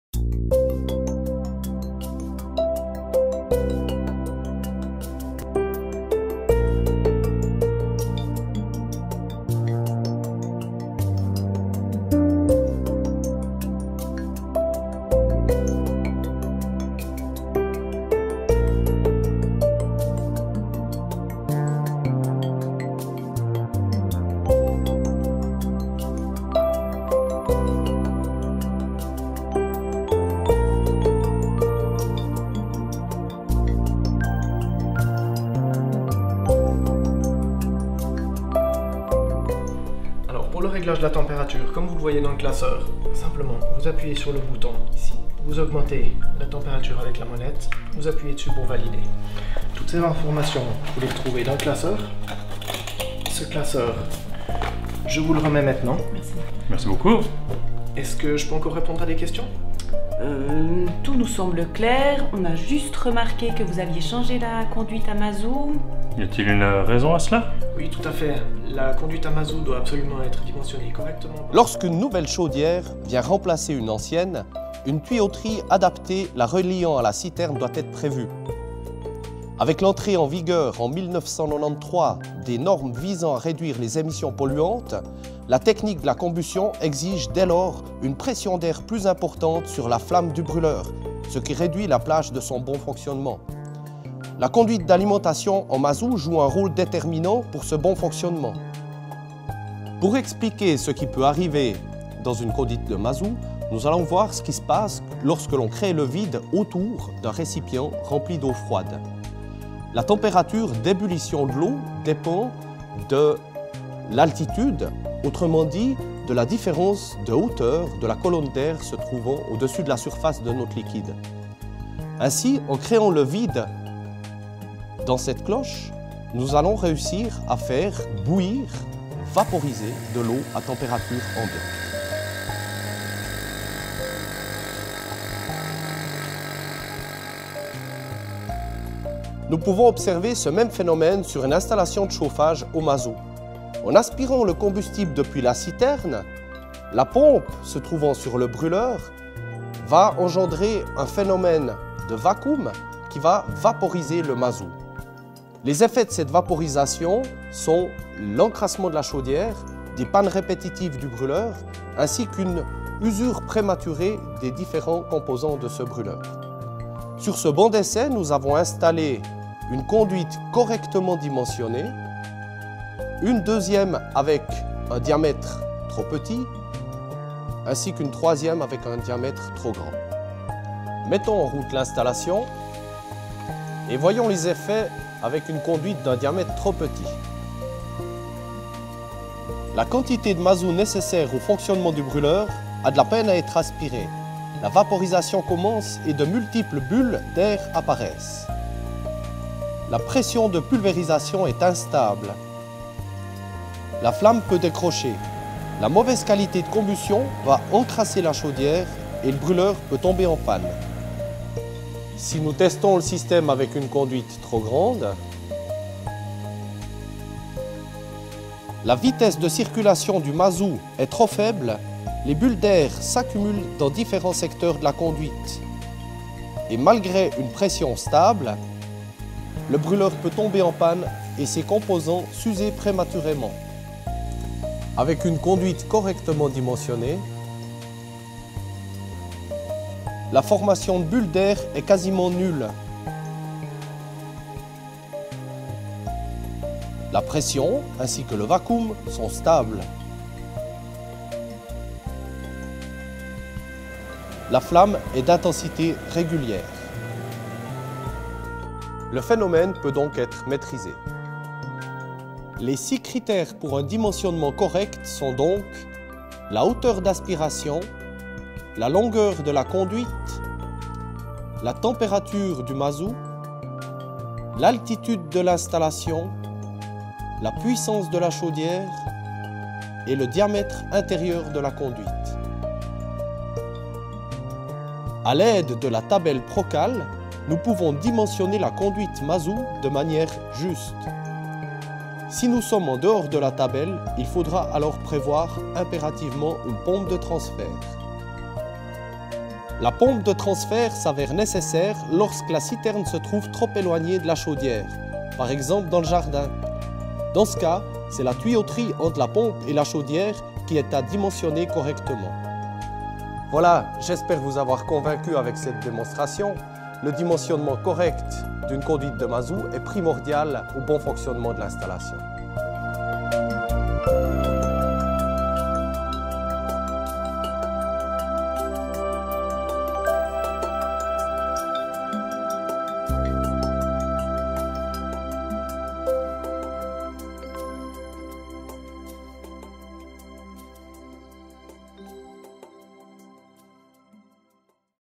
Assainissement des conduites de mazout, oui mais correctement!
Pourquoi est-il si important de remplacer les conduites de mazout lors de l'assainissement d'une chaudière?
Vous verrez ici très clairement les répercussions que peuvent avoir des conduites de mazout mal dimensionnées ou non assainies sur la sécurité de fonctionnement de votre chauffage.